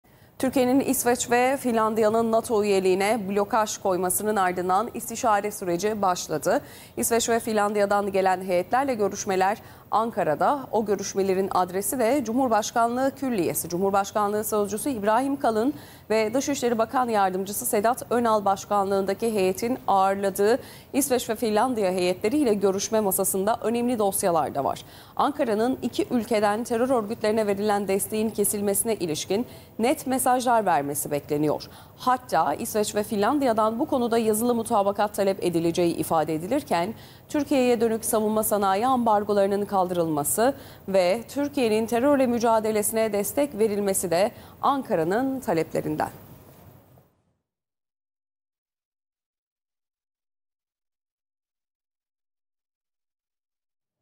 İsveç ve Finlandiya ile görüşmeler başladı
İsveç ve Finlandiya’nın NATO’ya üyelik başvurularına ilişkin istişare toplantısı Ankara Beştepe'de bulunan Cumhurbaşkanlığı Külliyesi'nde başladı. Türkiye heyetine Cumhurbaşkanlığı Sözcüsü Büyükelçi İbrahim Kalın ve Dışişleri Bakan Yardımcısı Büyükelçi Sedat Önal başkanlık ediyor.